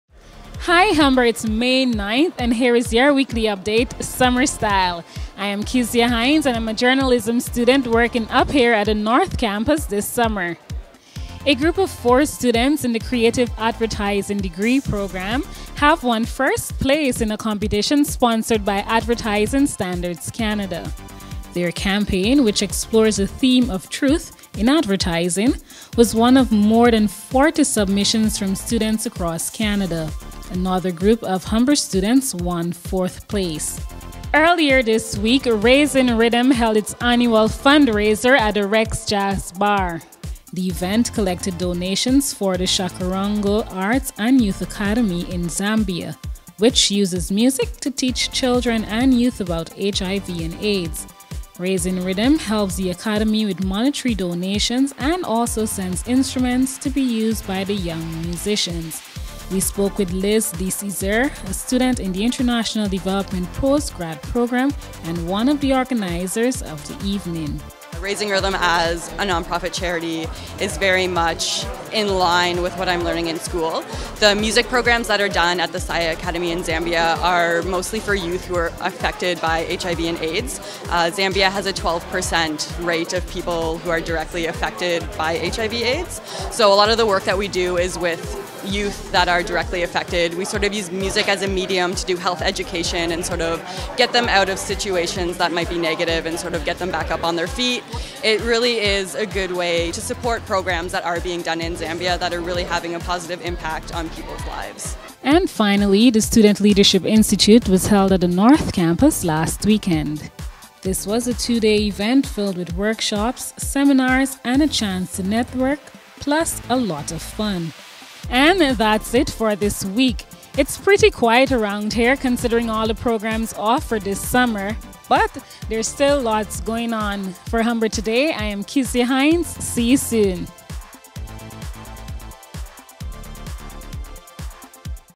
Advertising Competition, Raising Rhythm, Student Leadership : Humber Today
-Raising Rhythm held its annual fundraiser

-The Student Leadership Institute held a conference at the North Campus